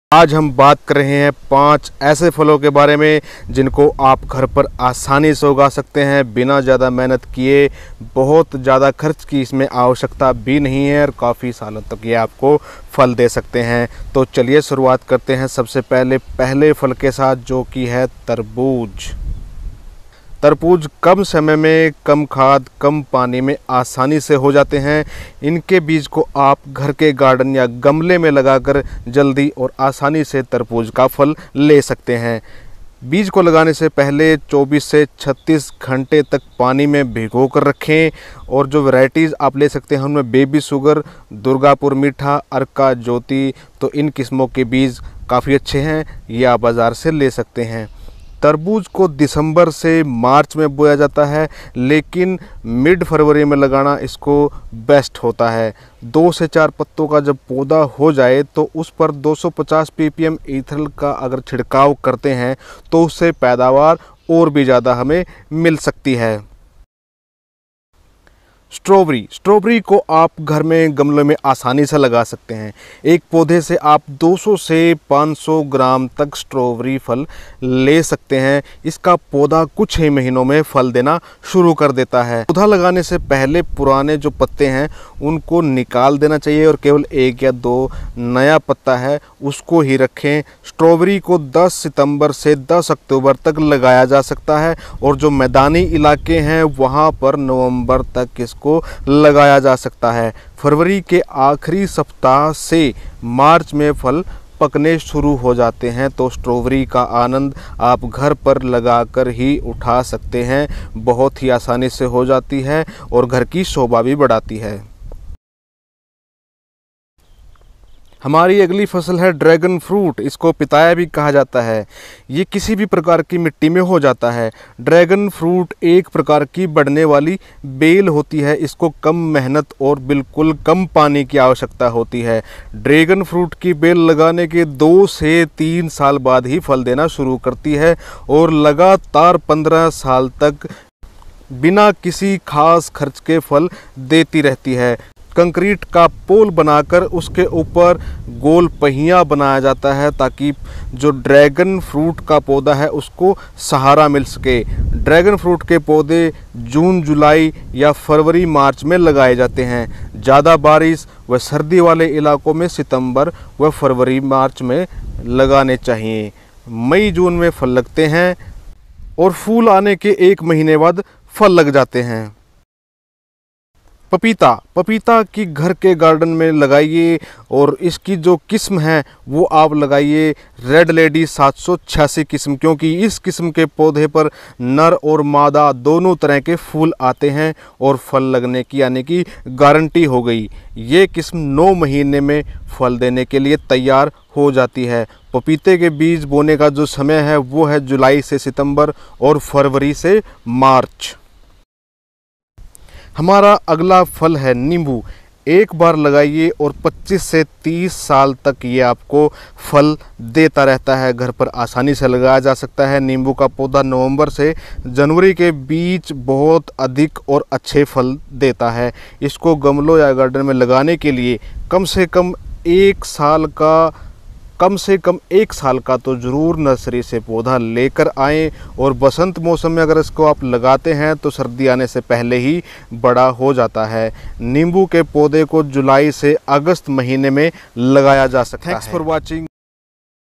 घर पर गमलों और गार्डन में आसानी से उगाये ये 5 फल | How to Grow Fruit Plants at Home
अपने घर गमलों और गार्डन में कैसे और कब उगाये ये 5 महत्वपूर्ण फल ।
ghar pr ugaye, घर पर फल, जैविक पर घर पर उगाये, जैविक फल के लाभ, How to grow, how to grow fruits plants at home, jevik kheti, organic farming, organic cultivation,